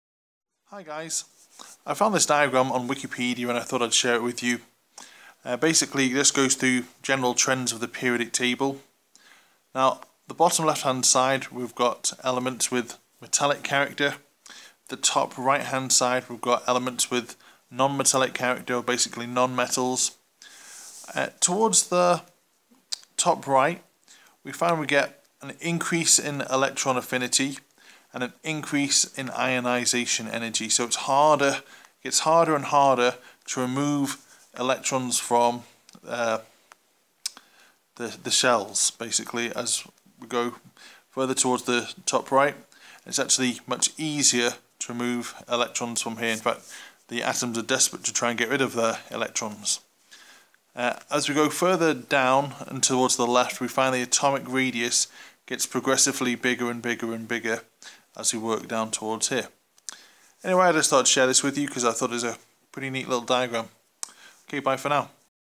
Trends in the Periodic Table - A level Chemistry
I found this nice diagram on Wikipedia explaining the general trends in the periodic table. I will make a second video explaining these trends in the future.